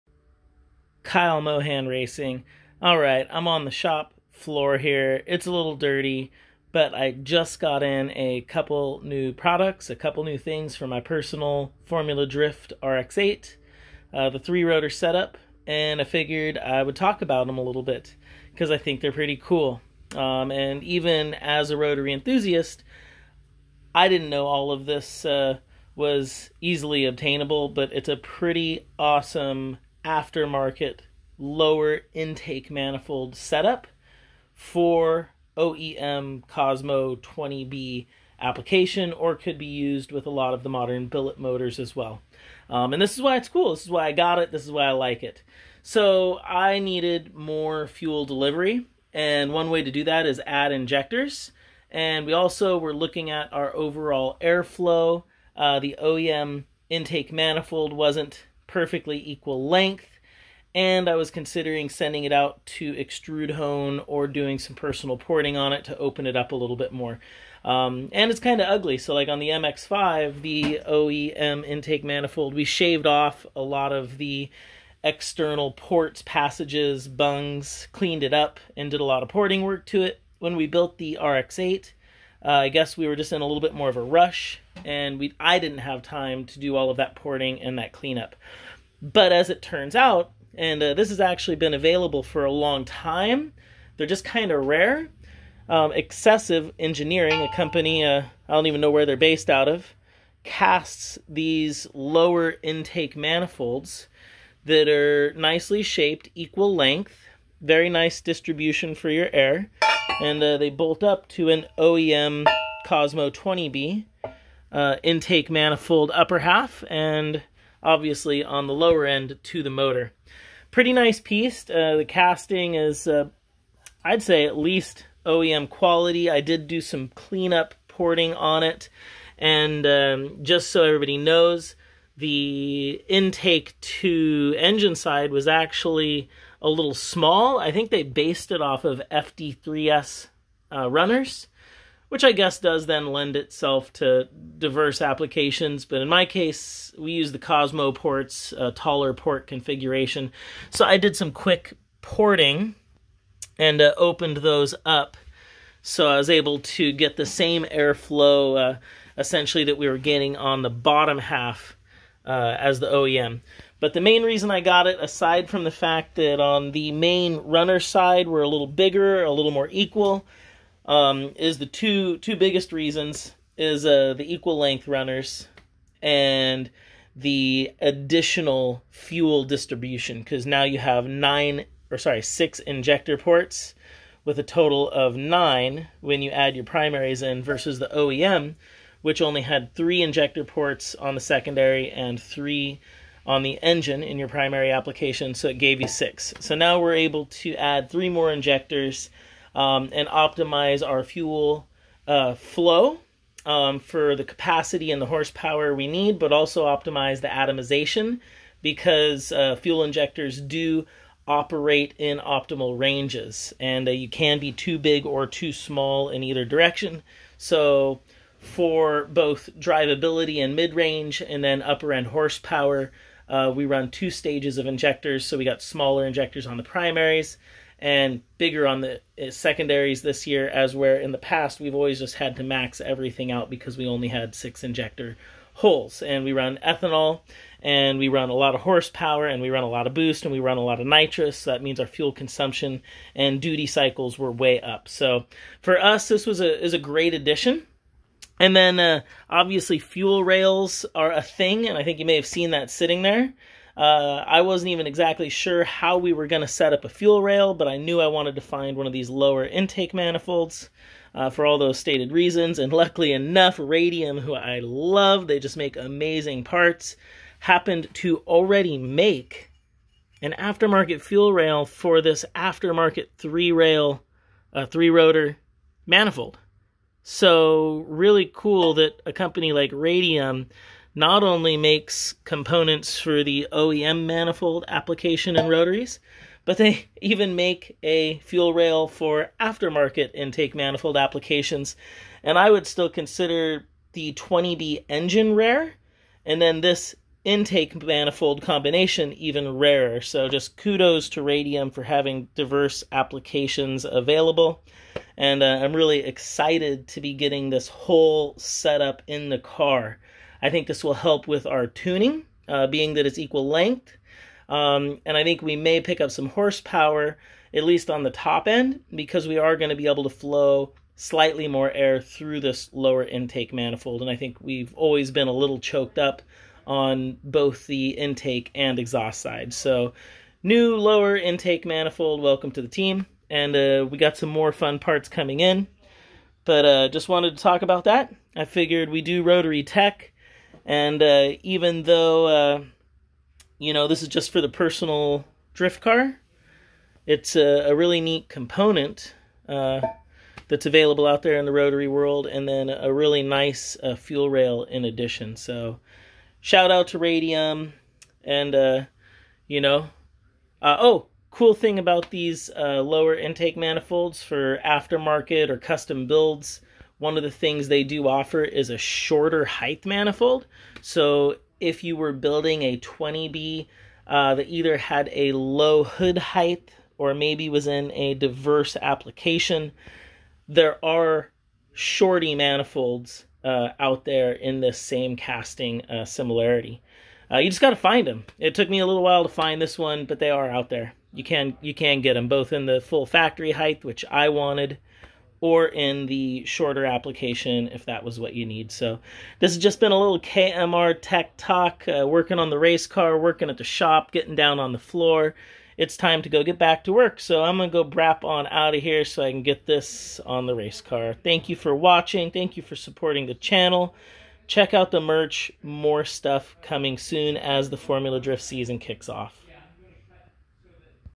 Custom 20b Lower intake manifold and Radium fuel rail setup / rotary tech talk
Talking about lower intake setups and fuel rails.